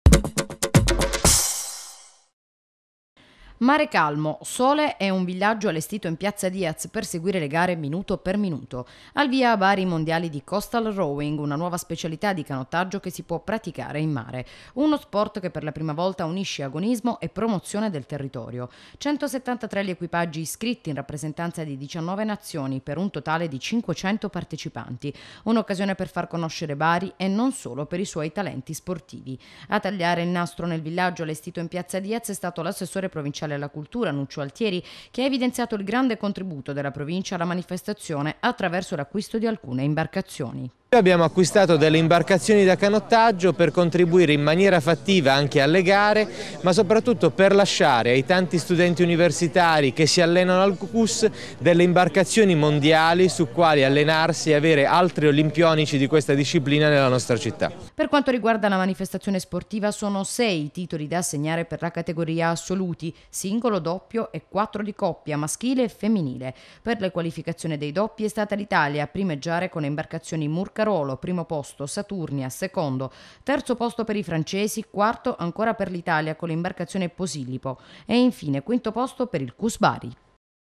TG 21.10.11 Al via a Bari i mondiali di Coastal Rowing
Al via da oggi a Bari i mondiali di Coastal Rowing. Per le qualificazioni dei doppi è in testa l'Italia. Una manifestazione che unisce sport e promozione del territorio.